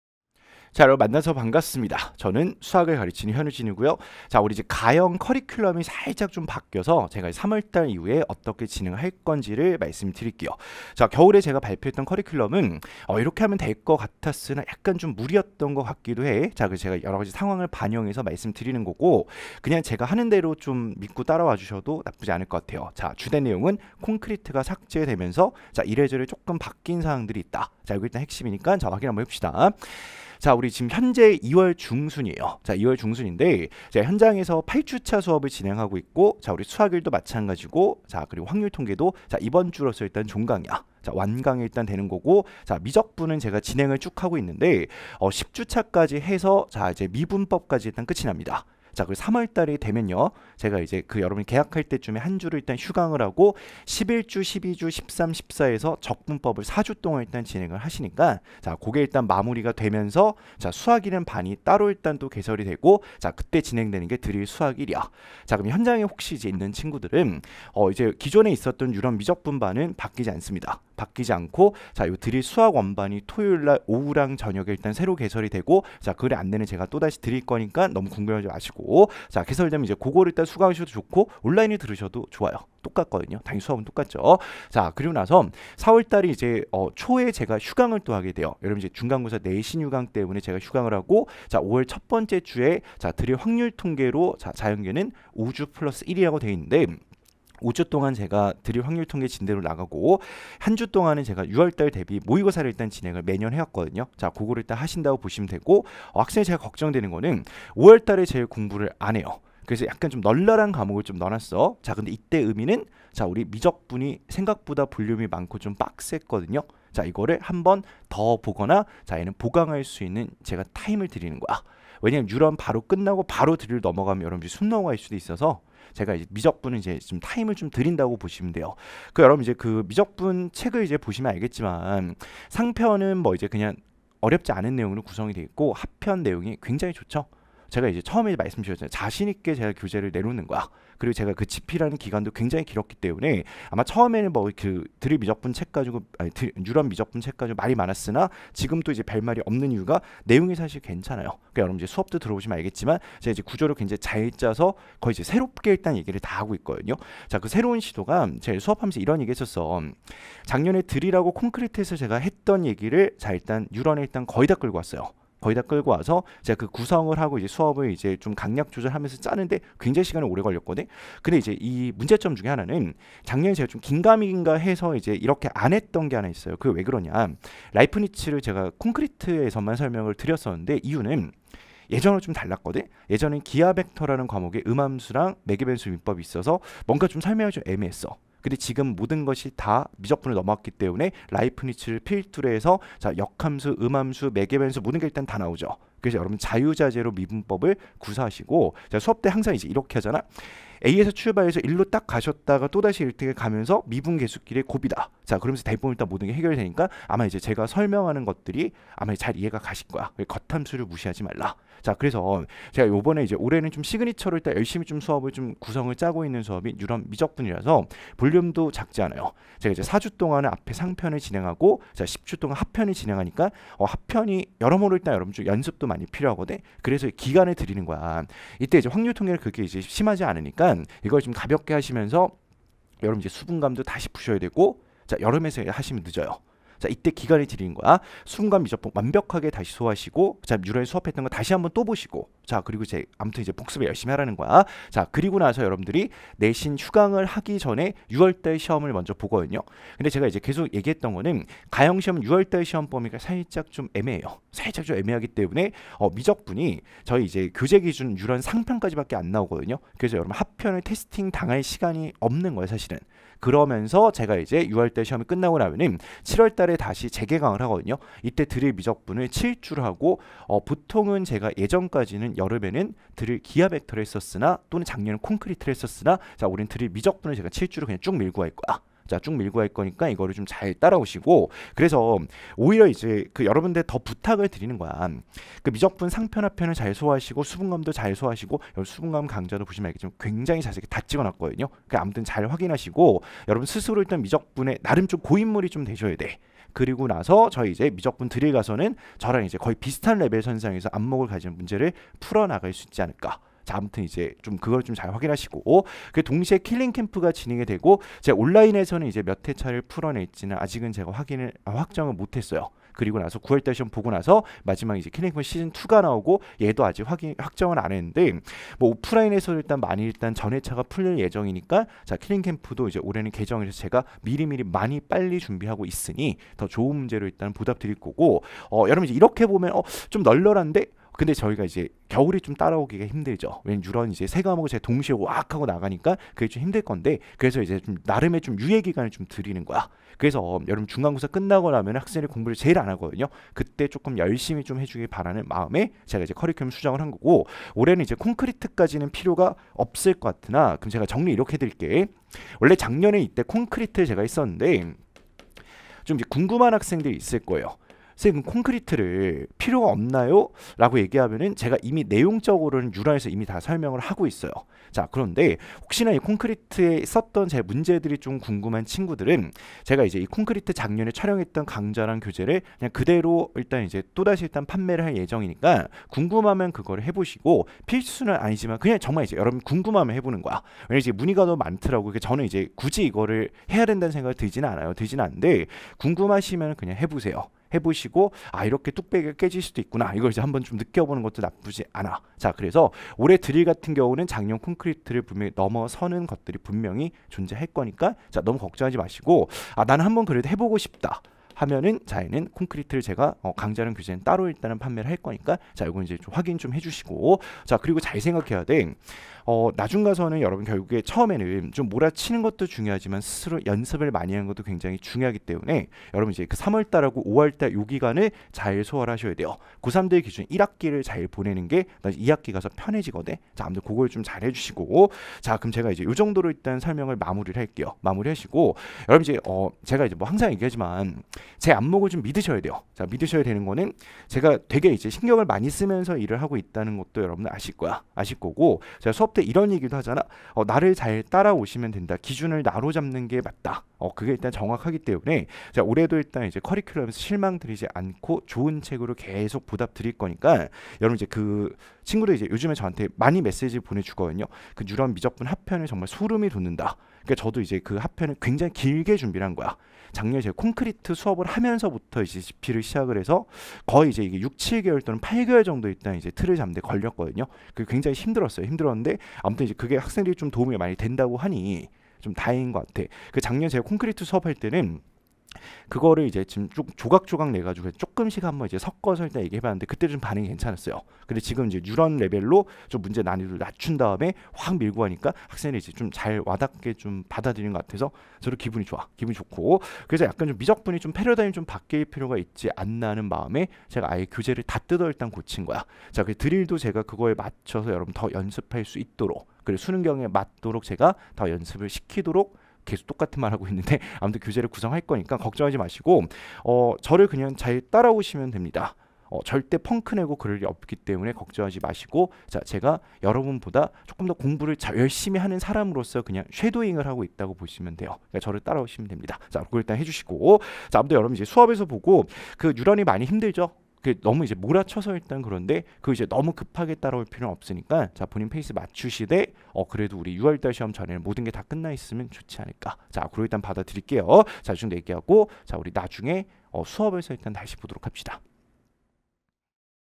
[메가스터디] 수학 현우진 쌤 - 3월 이후 2021 커리큘럼 가형
[2021VER  커리큘럼 가이드 가형]
 - 3월 이후 강좌 개강 안내

3월 : 뉴런 미적분 & 드릴 수학l
5월~6월 : 드릴 확률과통계
7월~8월 : 드릴 미적분 & 킬링캠프 시즌1
9월~10월 : 킬링캠프 시즌2

저를 믿고 따라오시면 됩니다.
현우진의 안목을 믿으십시오.

현우진 선생님에 대해 더 알고싶다면?